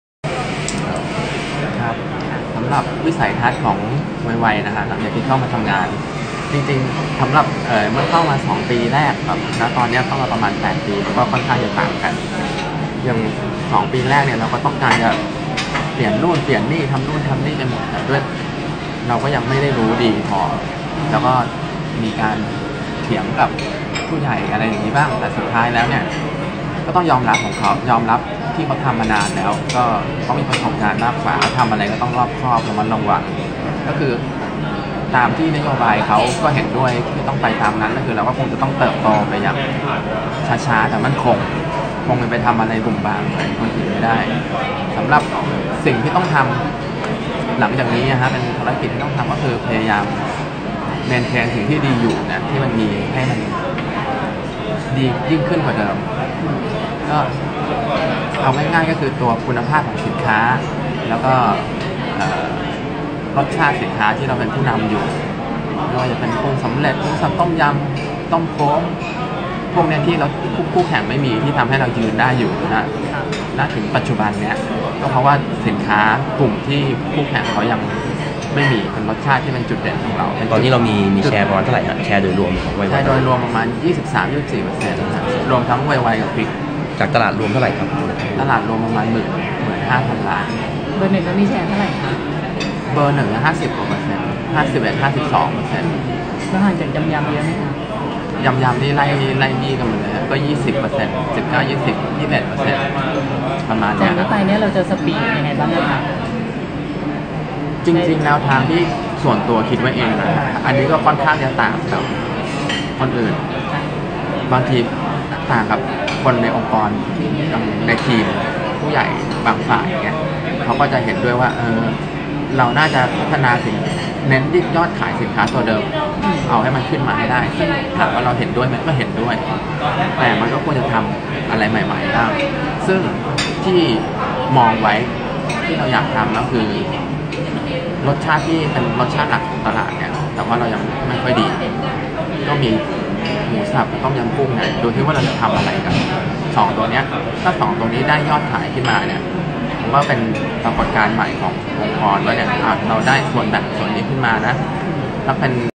“ไวไว”ลุยบะหมี่เกาหลีดันยอด ขยายฐานตปท.-โหมแบบถ้วย
2017/08/28 “ไวไว”ลุยบะหมี่เกาหลีดันยอด ขยายฐานตปท.-โหมแบบถ้วย